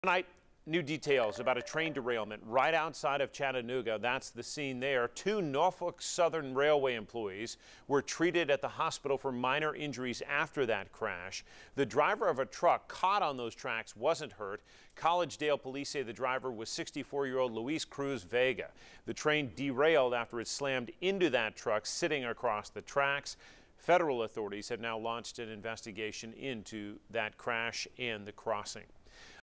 Smash-up on the tracks: Train slams into truck, derailing cars in TN town
The collision occurred in Collegedale near Chattanooga.